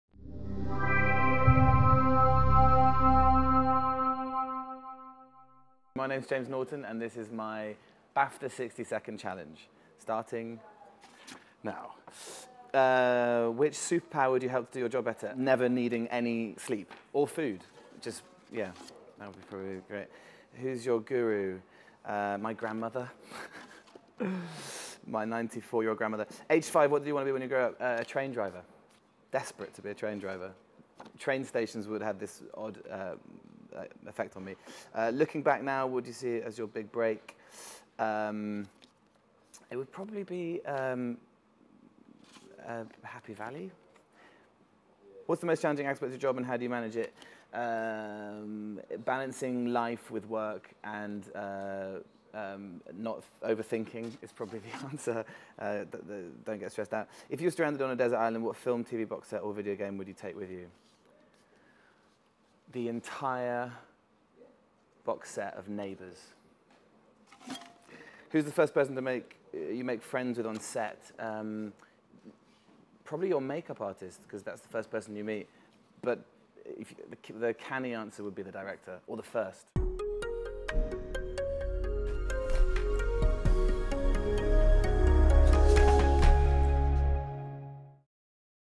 60 Seconds With...James Norton
War and Peace, Happy Valley and Grantchester actor James Norton takes on the BAFTA Guru 60 Second Challenge! Find out what he wanted to be aged 5, what he considers his big break, and the most challenging aspect of his job.